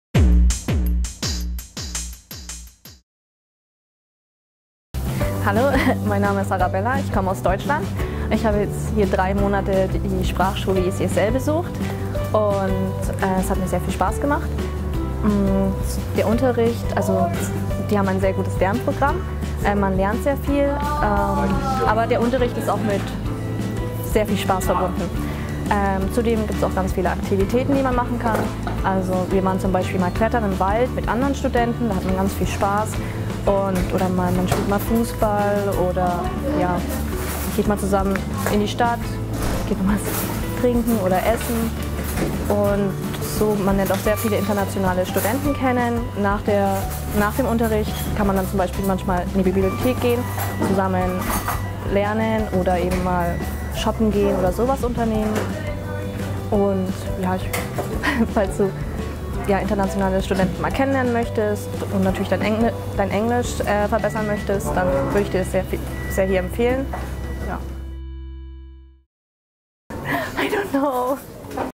Arabella's Testimonial
Checkout this testimonial of ECSL student Arabella.